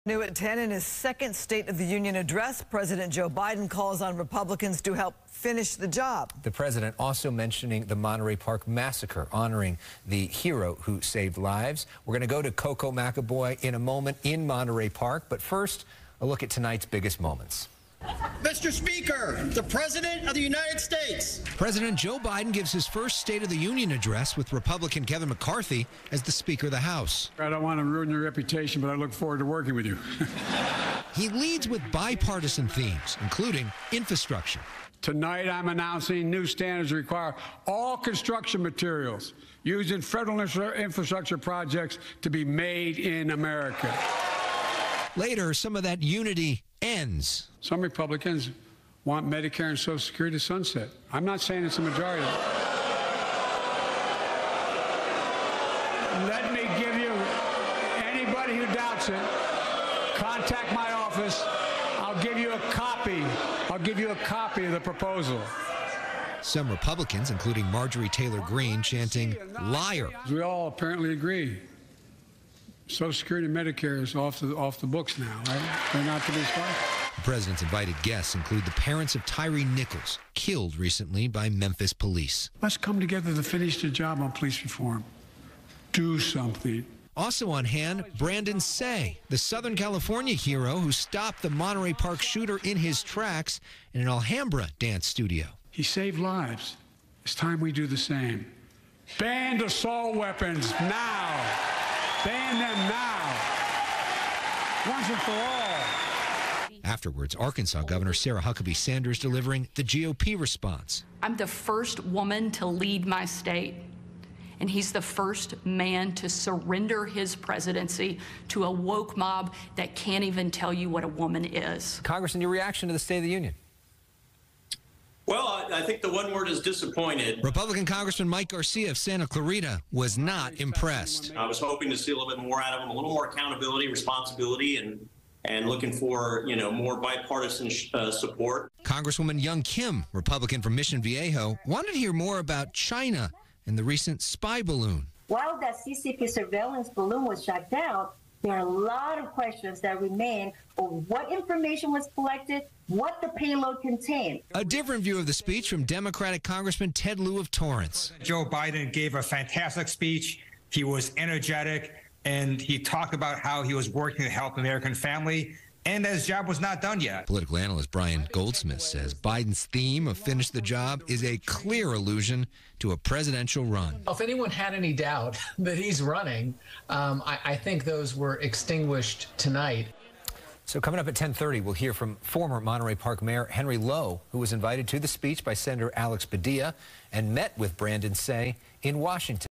Highlights of Pres. Biden's State of the Union 2023
The biggest moments & reaction to Pres. Joe Biden's second State of the Union Address.